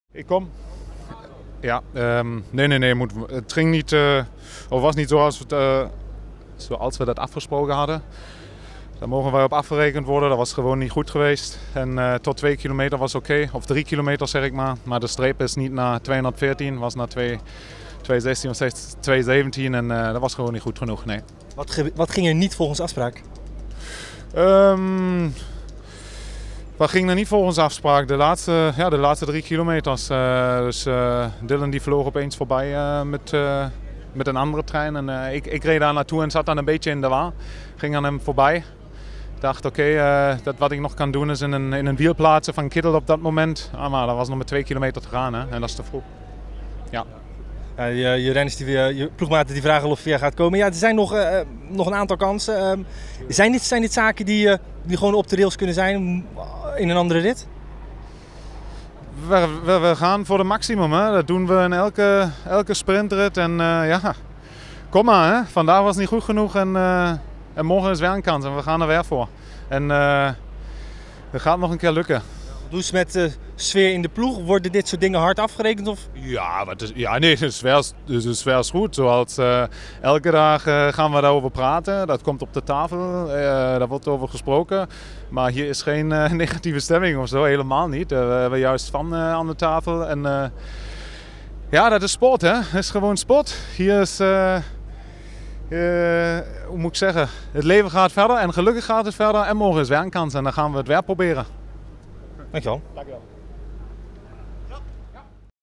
Robert Wagner: "Het ging niet zoals we afgesproken hadden" - WIELERFLITS
Dylan Groenewegen eindigde in de massasprint in Troyes als zesde. De sprinter van LottoNL-Jumbo zat in de ultieme finale zonder ploeggenoten, en daar mogen de renners op afgerekend worden volgens Robert Wagner. "Het ging niet zoals we dat afgesproken hadden", zegt hij voor de camera van WielerFlits.